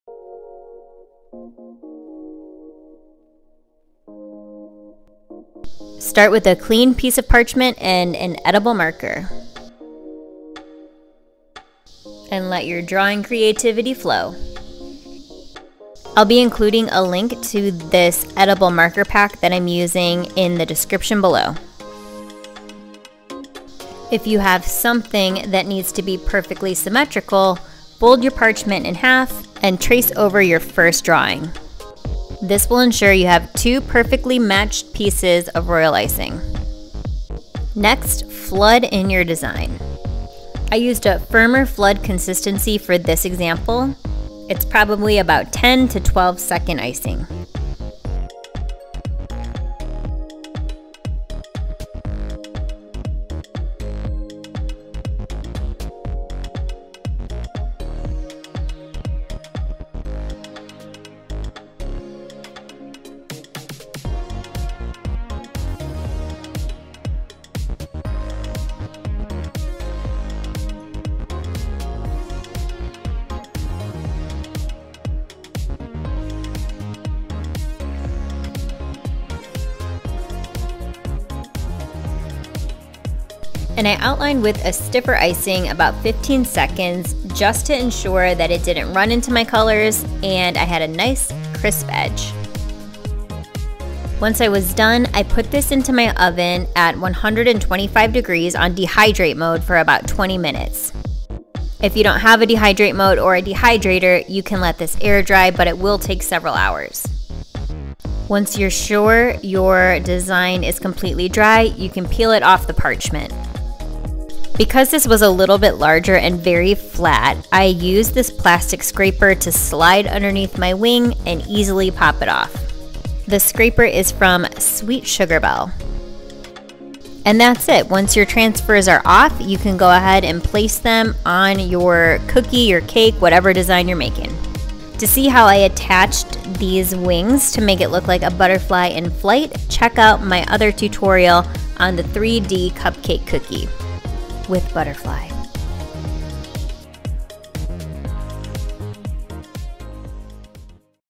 Making Royal Icing Transfers
This is a quick video on how I make my royal icing transfers. I also like to call them “appliqués” sometimes because I think it sounds fancy but I also think that is just a term for Jean art from the 70s… 🤷🏼‍♀️

I pipe with flood consistency and use the dehydrate mode on my oven set at 125 degrees for about 20 minutes.

The edible marker I use in the video comes in a multicolor pack. Check them out in this link:

Good luck with your appliqués! normalizeappliques 😂